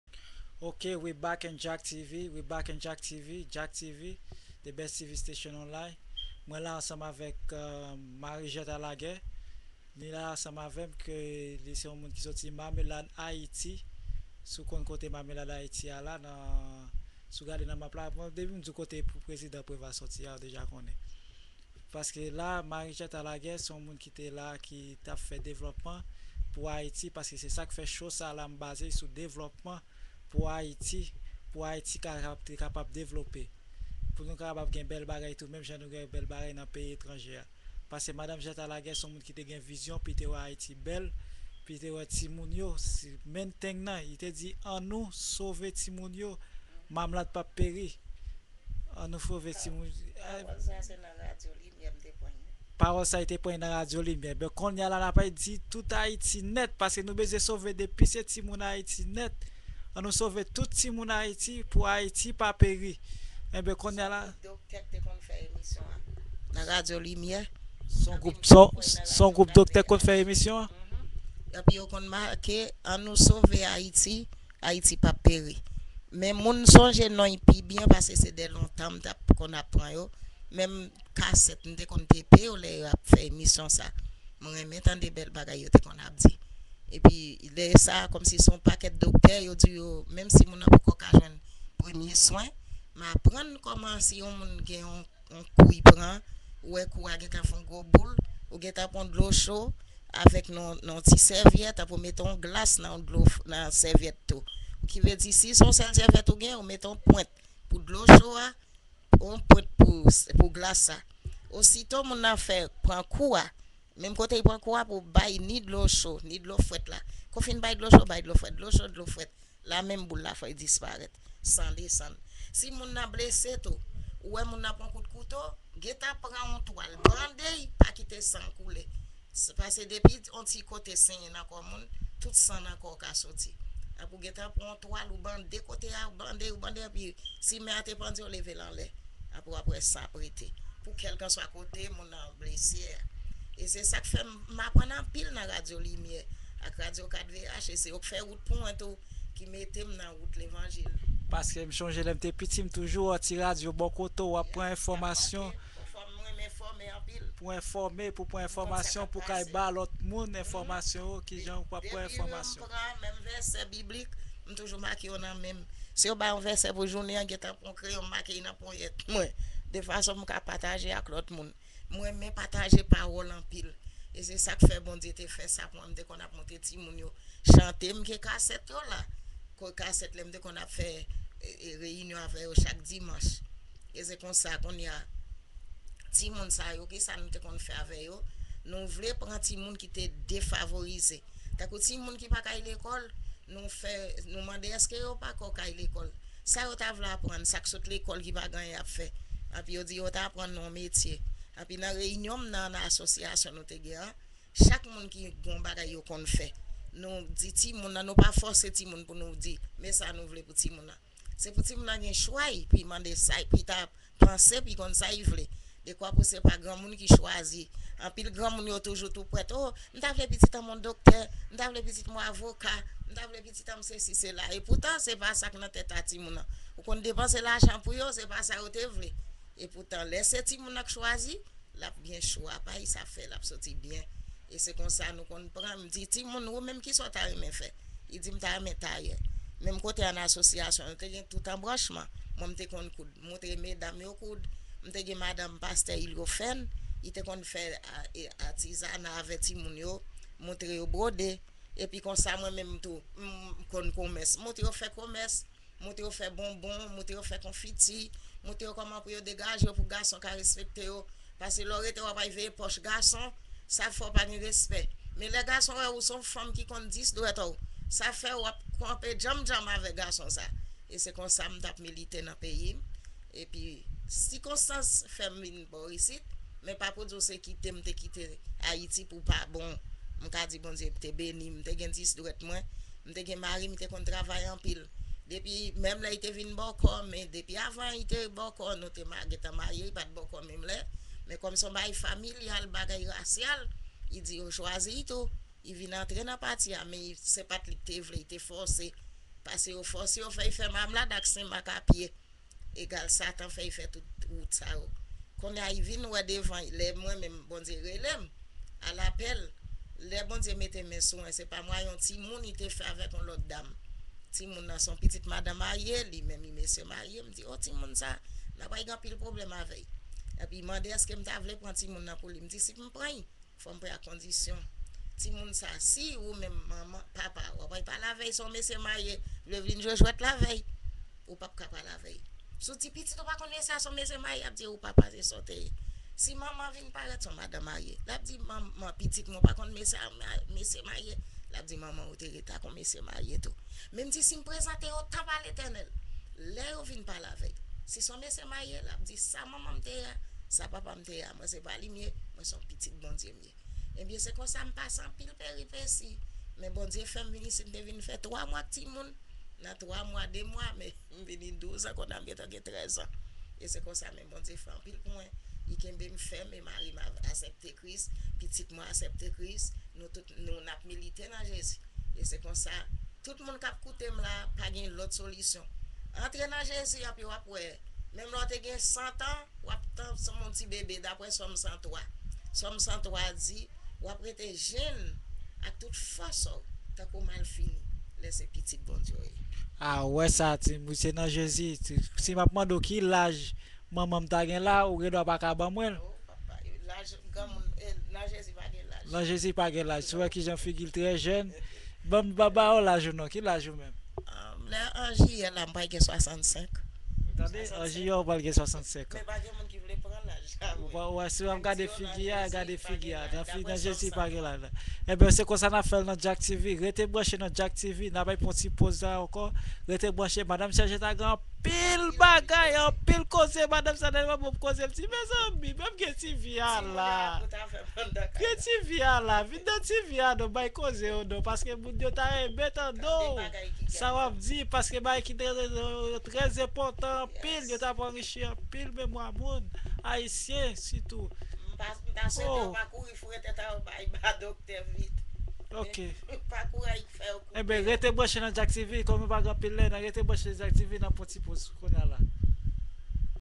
Marie G Laguerre in Jacktv part 2 know as madame Jacques Laguerre - March 11, 2009, 07:51 PM
jacklaguerre's QuickCapture Video - March 11, 2009, 07:51 PM
Marie G Laguerre doing bible study with the Haitian.